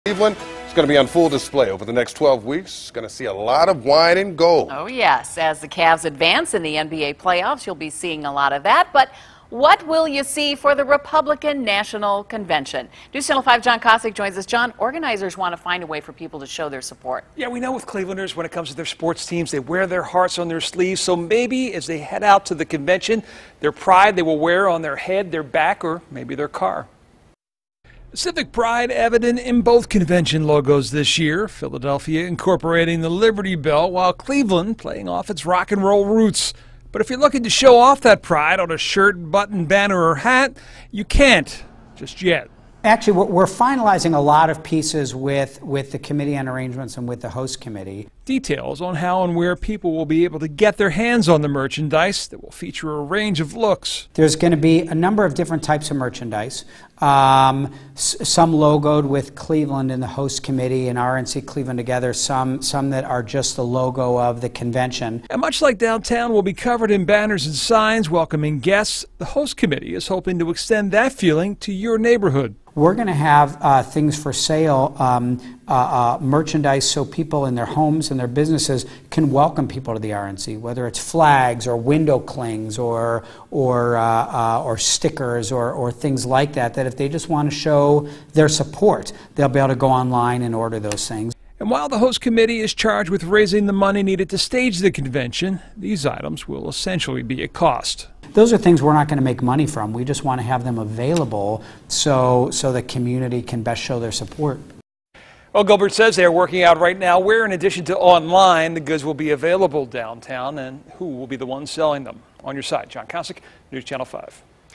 RNC Merchandise
Host Committee looks to share pride in Cleveland when merchandise for RNC is revealed. Live on Five